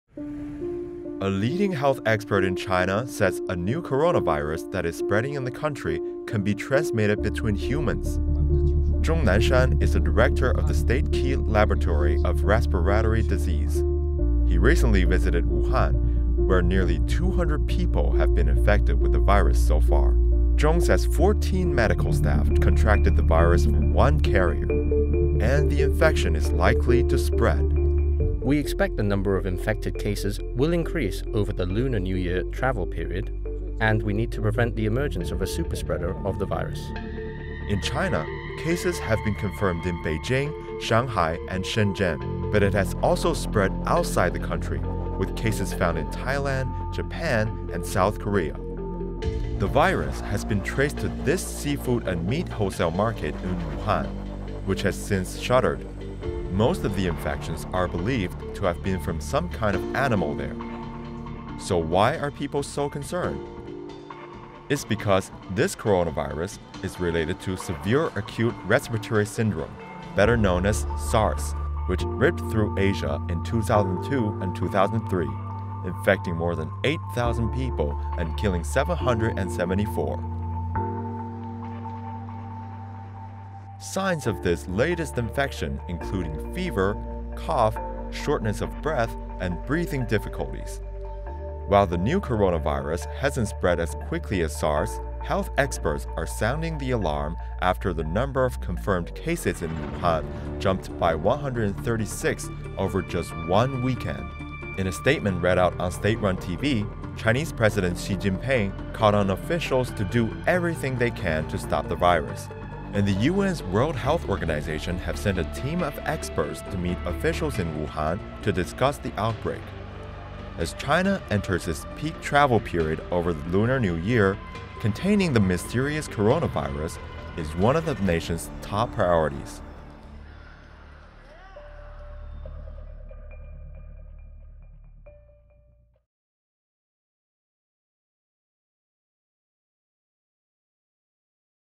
China confirms new coronavirus can be transmitted between humans
There is increasing concern over a new coronavirus that originated in the Chinese city of Wuhan, after officials confirmed cases of human-to-human transmission. The virus has killed at least six peopleand infected almost 300. But as China enters its peak travel season, hundreds of millions of people are rushing home for the Lunar New Year, leading health experts to predict the number of infections will rise. Chinese President Xi Jinping has meanwhile called on officials to do everything they can to contain the virus.